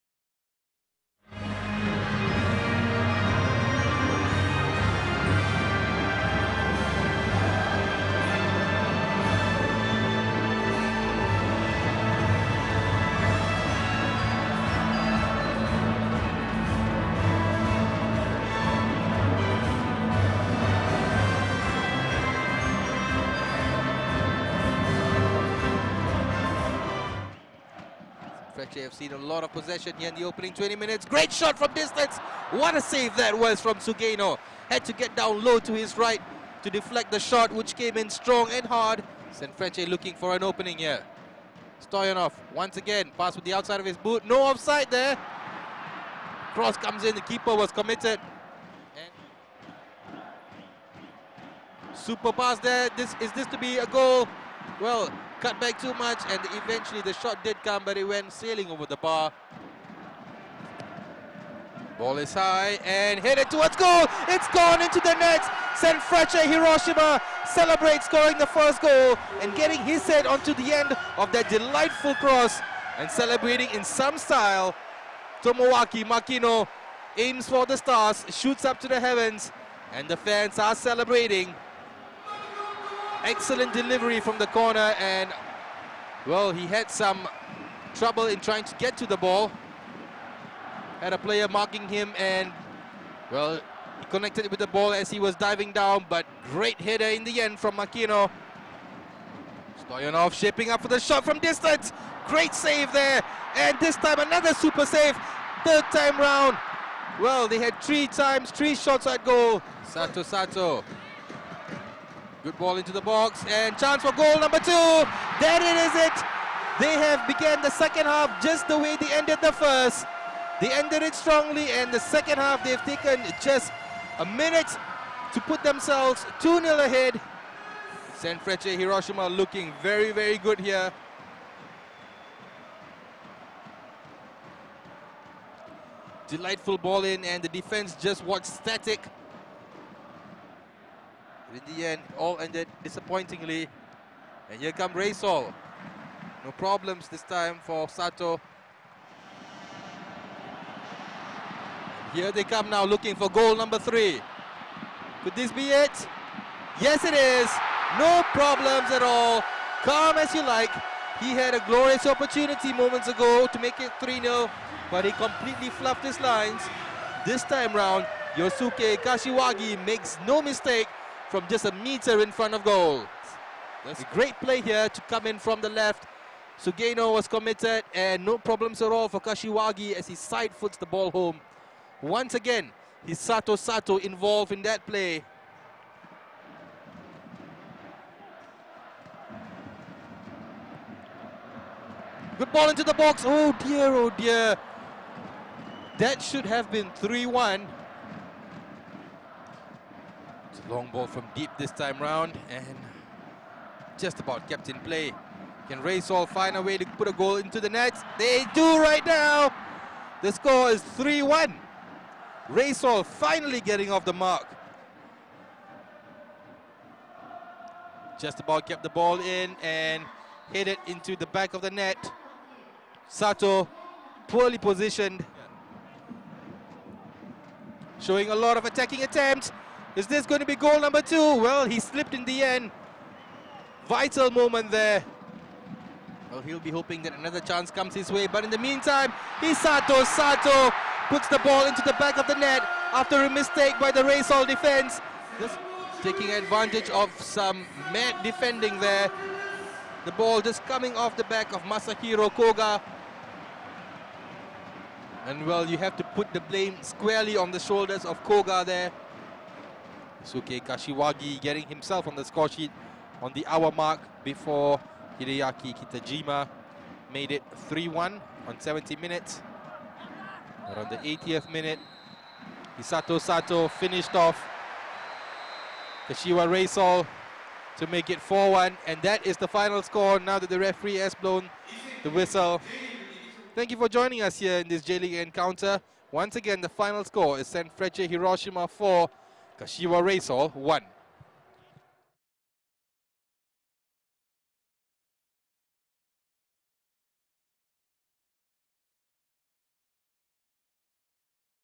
J League: Sanfrecce Hiroshima 4-1 Kashiwa Reysol 11/04/09 J-league
J League: Sanfrecce Hiroshima 4-1 Kashiwa Reysol 11/04/09 J-league Makino, Sato, Kashiwagi & Hattori score, watch extended higlights and live J-league